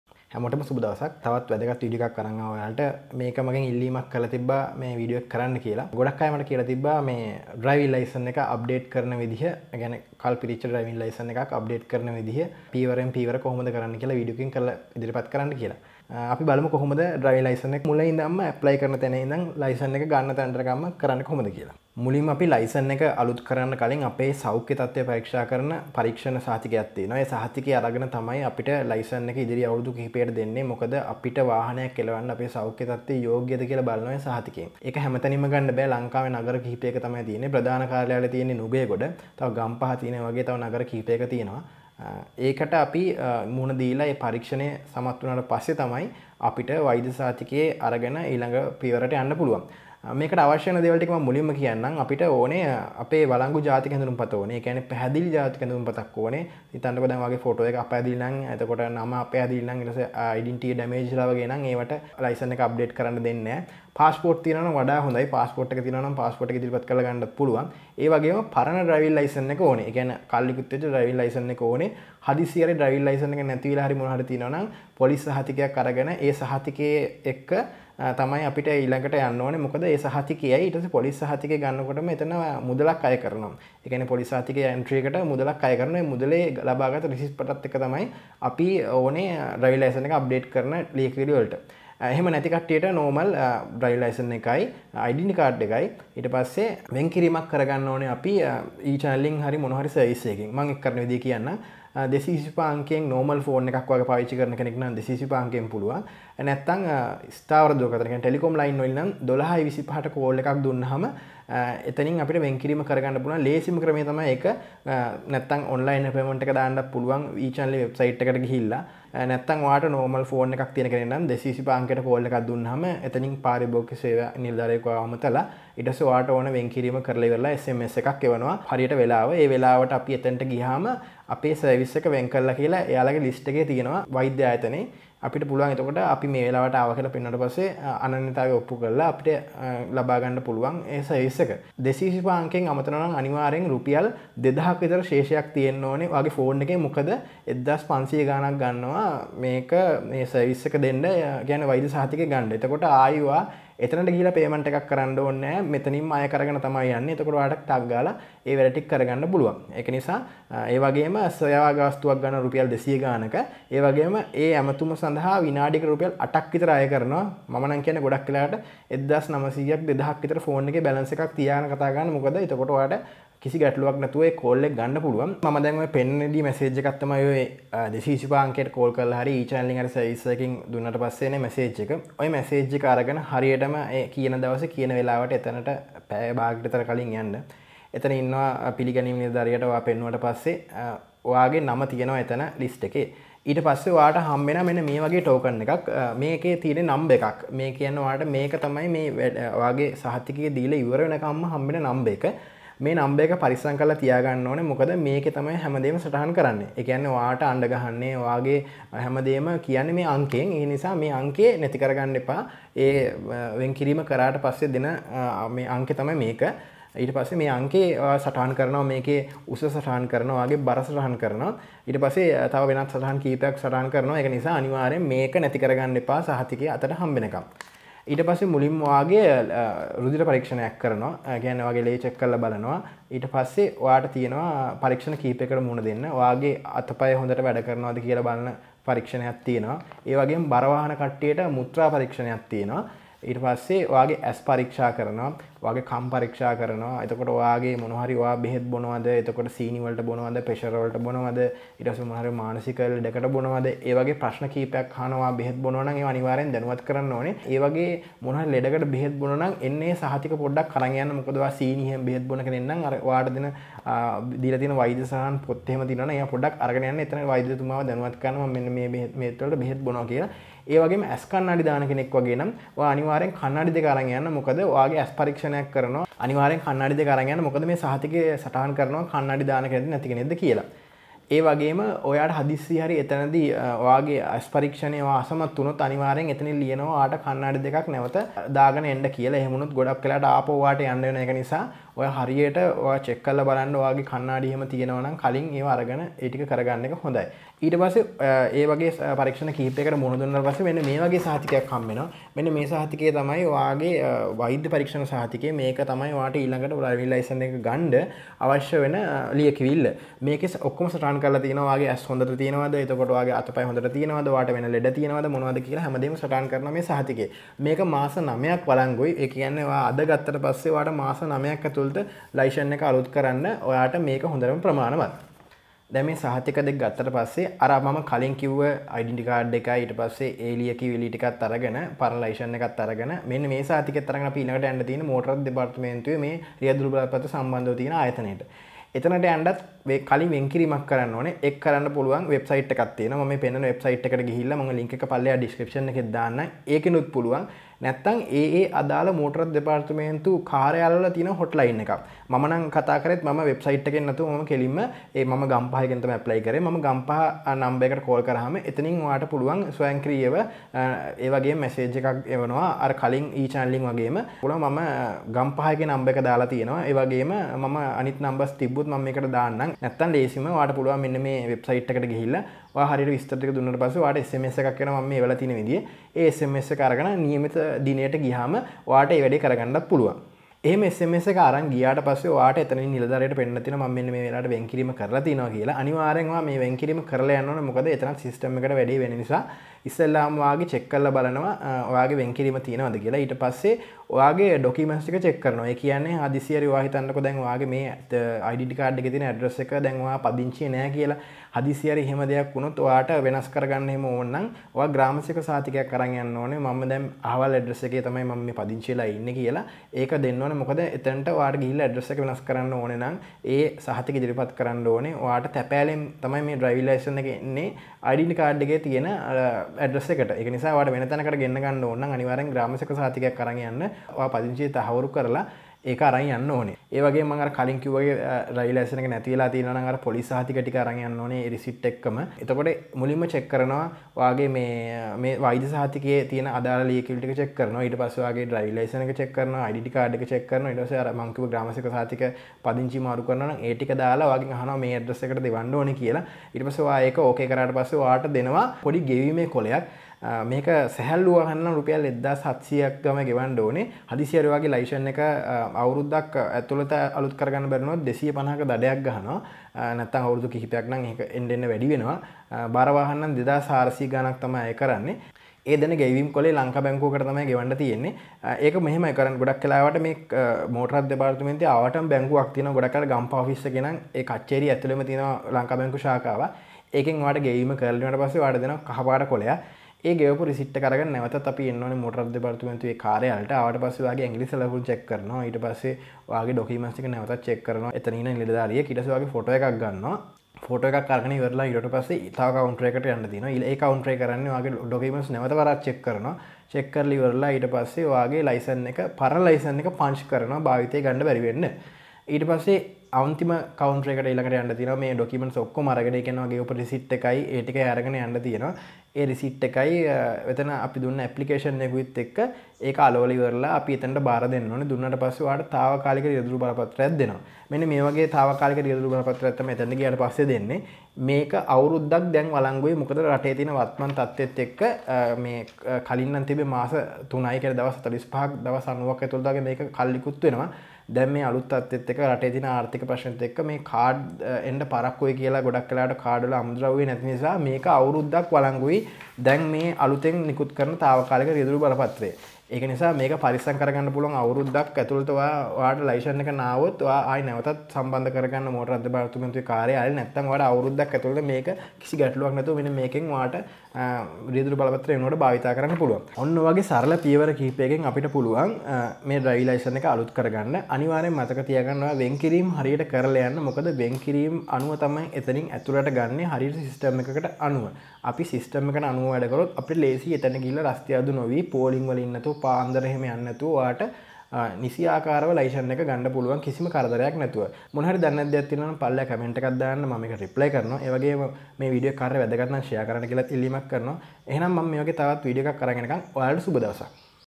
How to Driving License Renewal Srilanka | කල් ඉකුත් වූ ඩ්‍රයිවින් ලයිසන් එක අලුත් කර ගන්නා ක්‍රමය
කල් ඉකුත් වූ ඩ්‍රයිවින් ලයිසන් එක අලුත් කර ගන්නා ක්‍රමය | How to Driving License Renewal Srilanka

▶️ Call 225 (mobile) 1225 (Land Lines)

      Motif Digital Agency.